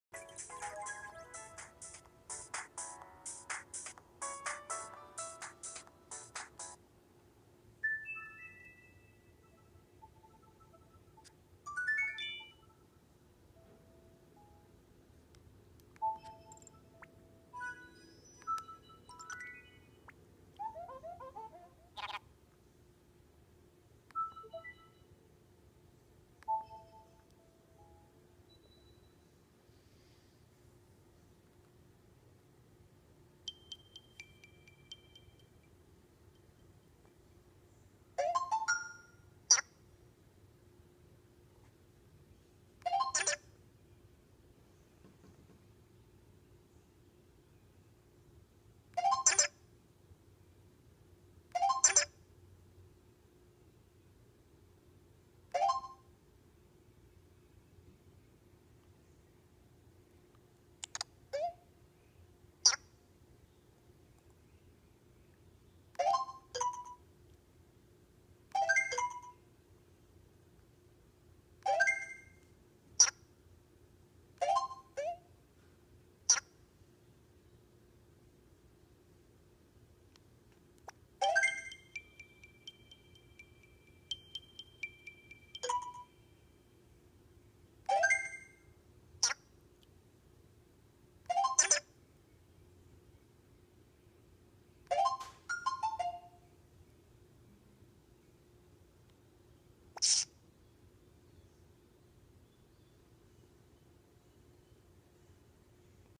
Flipnote Studio 3D!!
Hi guys! I just recently downloaded FNS3D from Club Nintendo! I was very excited about this when I heard it came out. I might even post some of my Flipnote Animations!